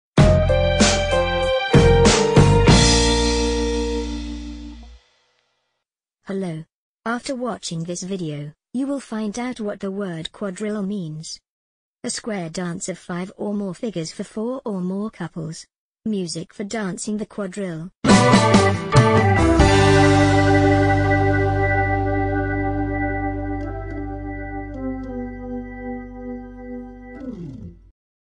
Definition of the word "Quadrille"
you will find full information about this word "quadrille".
From the video you will learn the definition for the word quadrille. The lexical meaning of the word quadrille. What does the word quadrille mean? And much, much more...